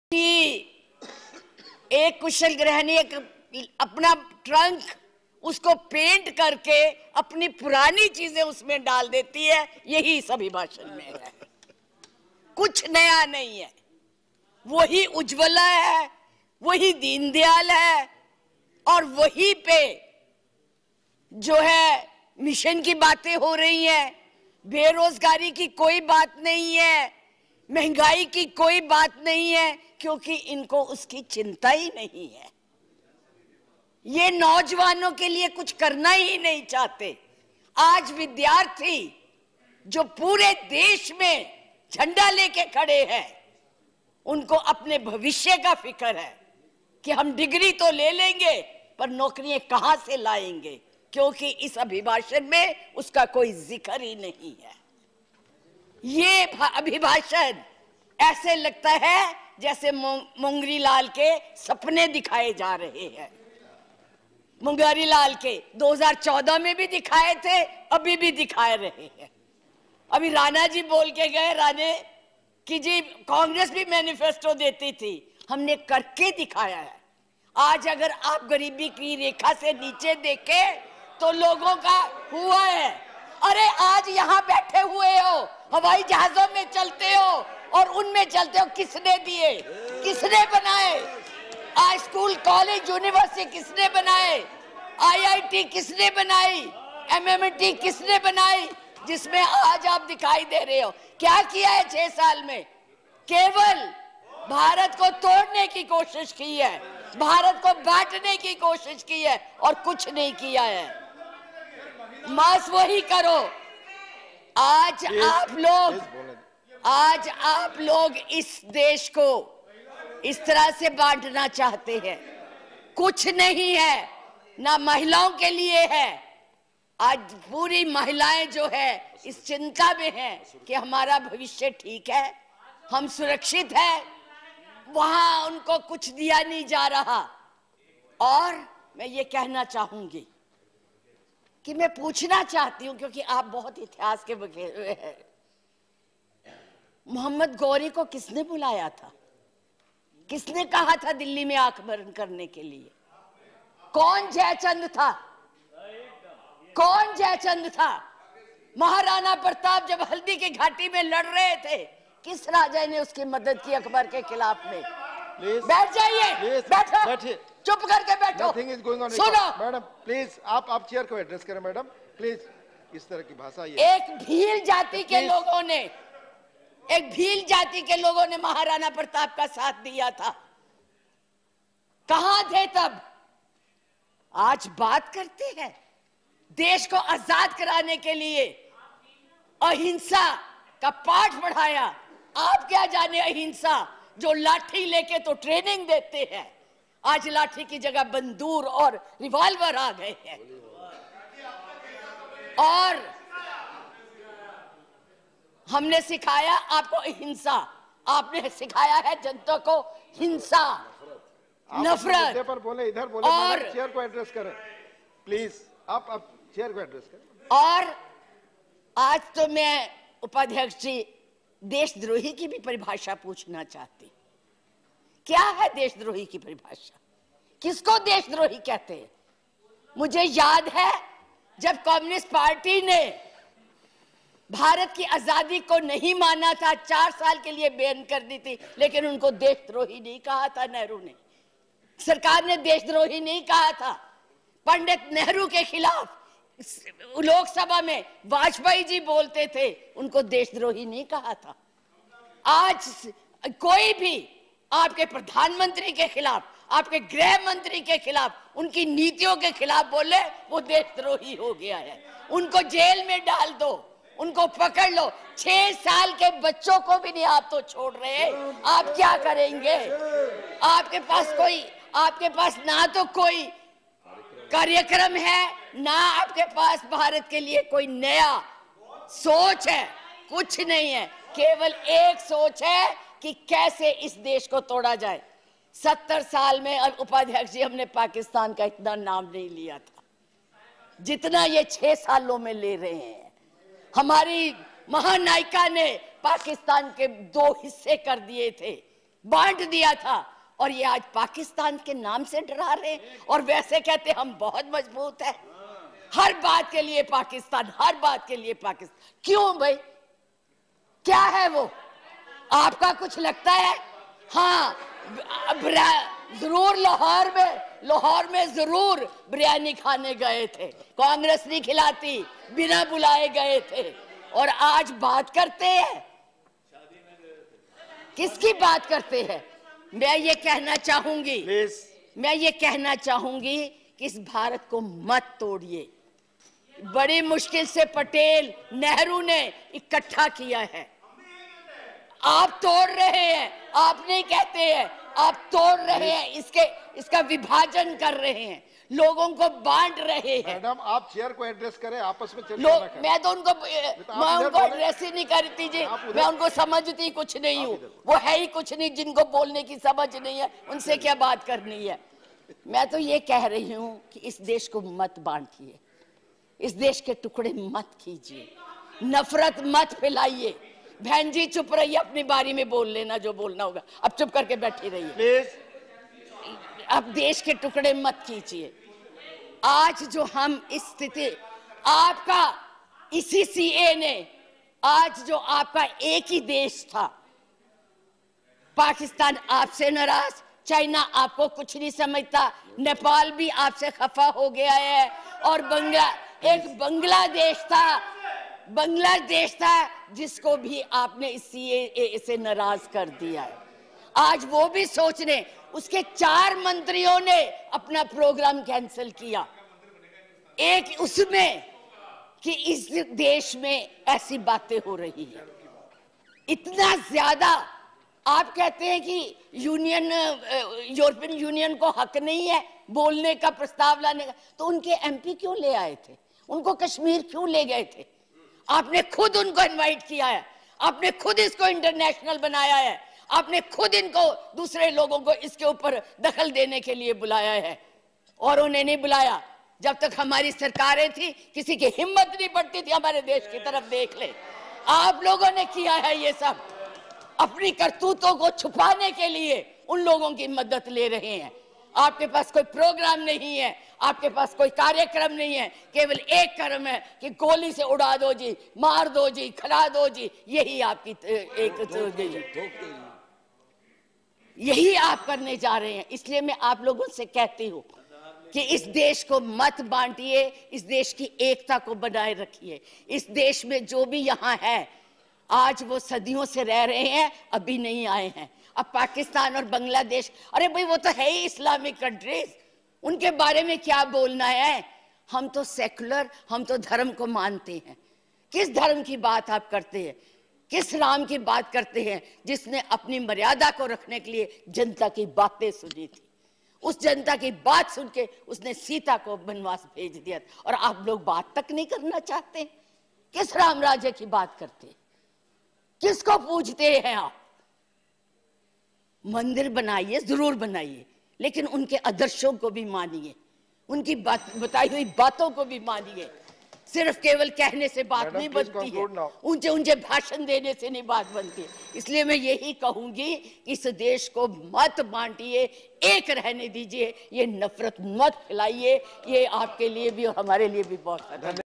कांग्रेस सांसद विप्लव ठाकुर ने मोदी सहित पूरी बीजेपी को धा डाला
देश बड़ी मुश्किल से बना है इसे तोड़िए मत, इसको जोड़ना का काम करिए